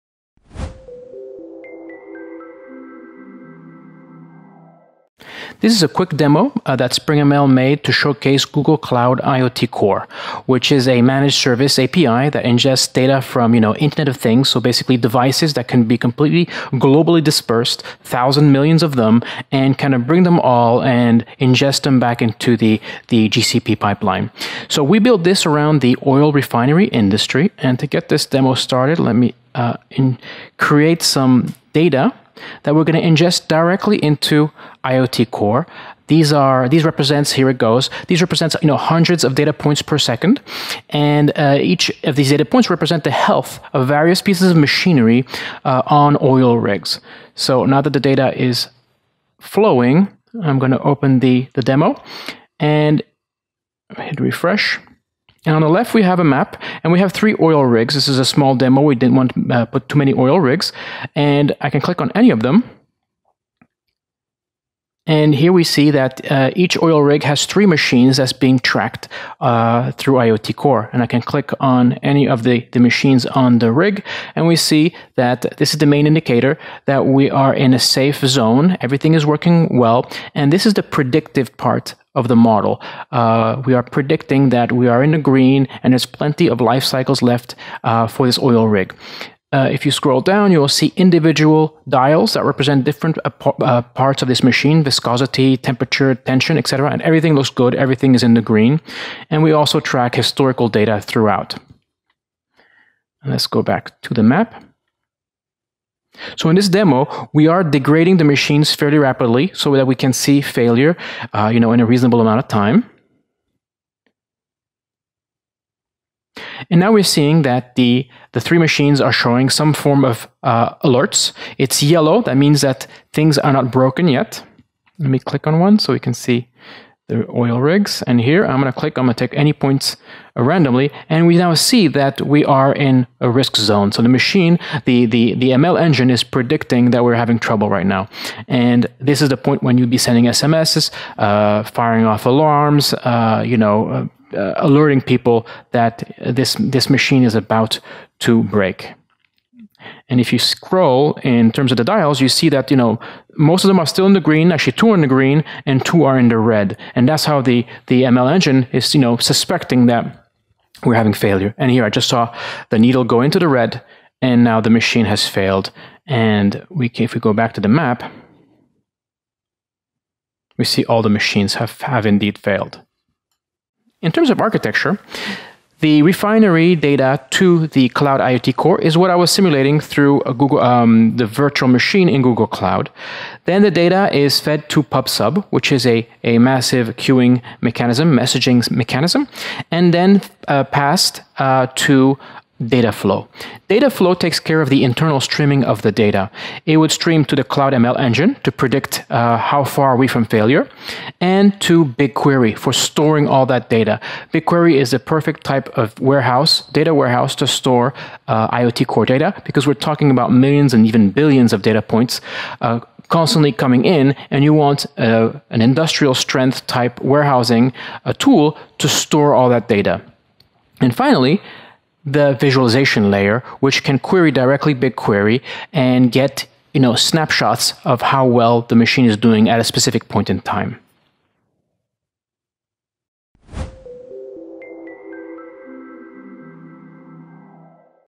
Google IOT Core - Preventive Maintenance
In this demo, we will cover use of Google IOT Core to build preventive maintenance.